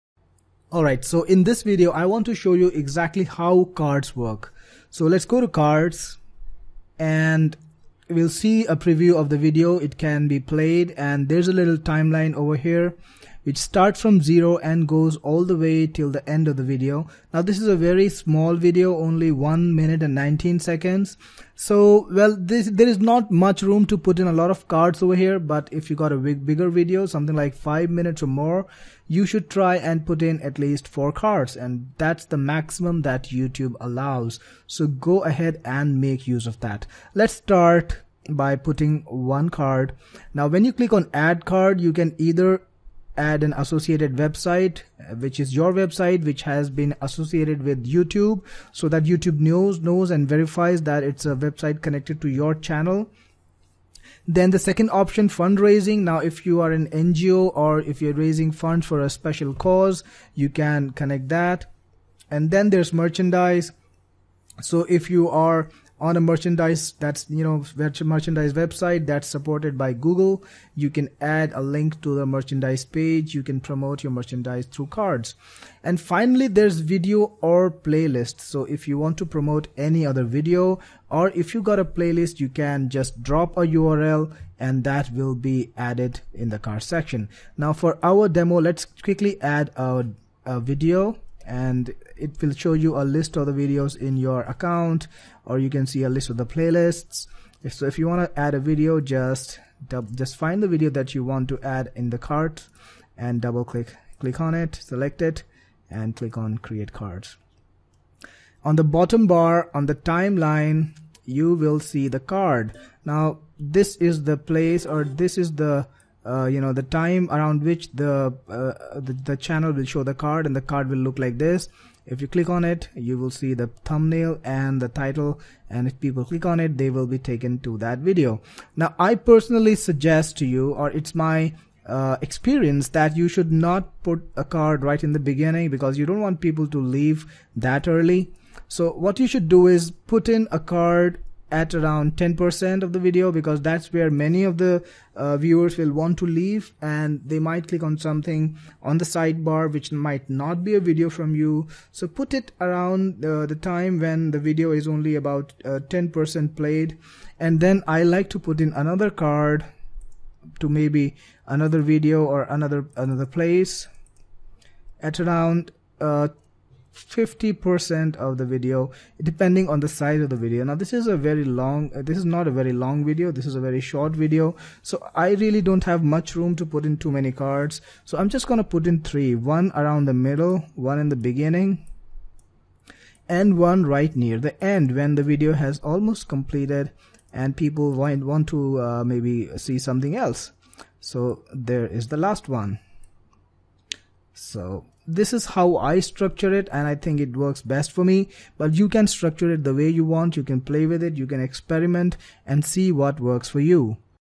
YouTube Ranking & Optimization Mastery - Be No.1 | Cards Demonstration
Welcome to the YouTube Channel Creator & Learn all secrets about YouTube Channel here !
YouTube Ranking & Optimization Mastery - Be No.1 | Engagement Techniques - Cards Demonstration

Thinking about building your YouTube channel into a business?
YouTube ranking factors : The top ranking signals to optimize for are:
Watch time:
Video length: